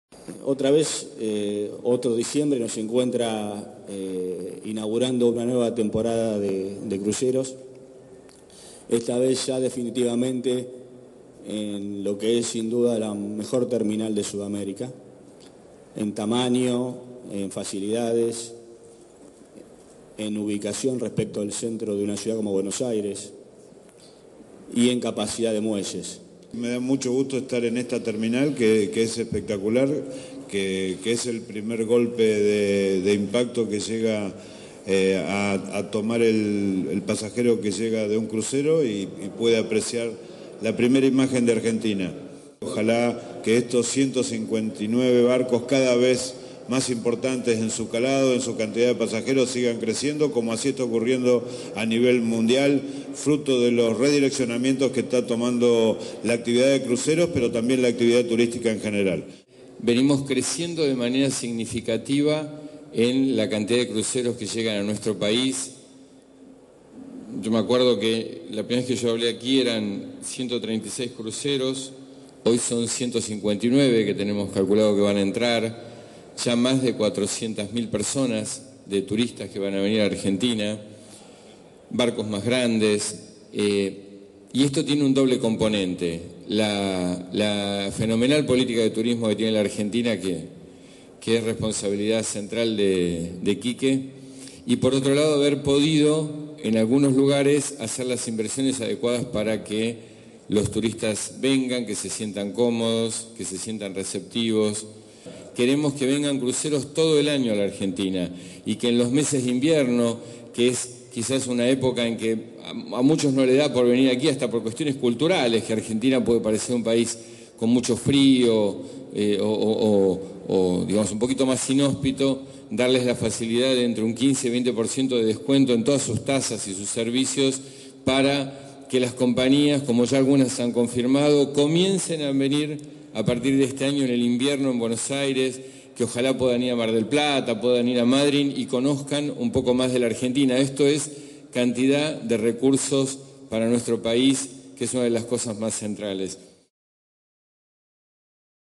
Esta temporada arribarán a buenos aires 159 cruceros con 420.000 turistas
Unos 159 cruceros y 420.000 turistas de todo el mundo arribarán al puerto de Buenos Aires, durante los meses de diciembre, enero y febrero, aseguraron hoy autoridades nacionales de turismo y transporte al inaugurar oficialmente la temporada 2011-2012 en la nueva terminal Quinquela Martín.
El ministro de Turismo, Enrique Meyer, expresó sus expectativas "por que estos 159 cruceros que van a ingresar al puerto de Buenos Aires en esta temporada sigan creciendo, como está ocurriendo a nivel mundial, fruto del redireccionamiento que está tomando la actividad y también el turismo en general".
Asimismo, destacó las características de la nueva terminal donde ingresan los cruceros porque "es el primer golpe de impacto, la primera imagen de Argentina para los visitantes".
La terminal cuenta con un edificio central de 12.300 metros cuadrados, una sala de espera para 2.000 personas, un depósito con capacidad para más de 12 mil valijas y 72 mostradores de chequeo y embarque, entre otras comodidades.
El lanzamiento de la temporada de cruceros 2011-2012 fue inaugurado hoy en el puerto porteño por el ministro de Turismo, el secretario de Transporte de la Nación, Juan Pablo Schiavi, el interventor en la Administración General de Puertos (AGP), Oscar Vecslir, y Gustavo Figuerola de la Terminal Río de la Plata.
Durante el acto, se firmó una resolución entre la Secretaría de Transporte y la AGP de bonificación tarifaria de tasas a los pasajeros de cruceros internacionales, con el objetivo de extender la llegada de cruceros más allá de la temporada estival.
Asimismo, se firmó entre el Ministerio de Turismo y la Terminal Río de la Plata, el convenio Marca País para incrementar y potenciar el nivel de las prestaciones y servicios que ofrece la terminal a los turistas.
Durante la presentación de la temporada, Schiavi expresó las expectativas del área sobre el crecimiento de la actividad de los cruceros internacionales, al resaltar que "pasamos en un año de 123 barcos a 159", y aseguró que en esta temporada "llegarán 420.000 turistas, no sólo de Europa sino también del turismo regional".
En este sentido, destacó que "estamos en una terminal de cruceros inaugurada hace menos de un año que tiene todas las comodidades para que barcos cada vez más grandes puedan venir a la Argentina, complementando este circuito que es Buenos Aires, Mar del Plata, Puerto Madryn, Ushuaia".
Schiavi se refirió a la firma de la resolución "por la cual vamos a bonificar a los barcos que vengan fuera de temporada, que termina en febrero, con un 20% en todos los servicios y las tasas".
Con este mismo espíritu, "estamos pidiendo a las Pymes, a las compañías, a las industrias argentinas, que nos animemos a vender productos y servicios de enorme calidad a estos cruceros internacionales, porque es una manera de posicionar la marca de nuestro país en el mundo", señaló.